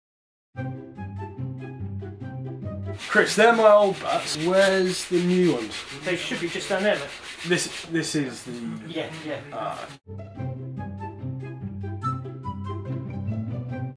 What's with Bilbo's Bats?
Sam Billings at Gray-Nicolls HQ in Robertsbridge, picking up new bats with a different kind of attitude. Find out Friday Teaser video